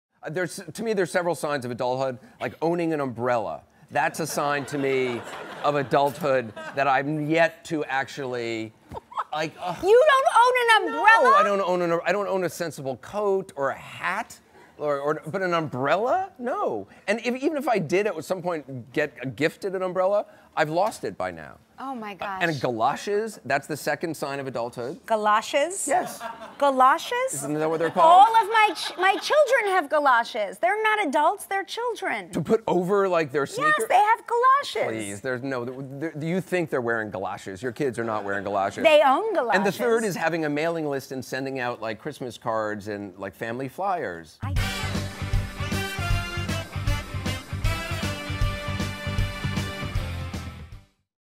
Signs of Adulthood
Anderson Cooper tells Kelly what he thinks are signs of adulthood.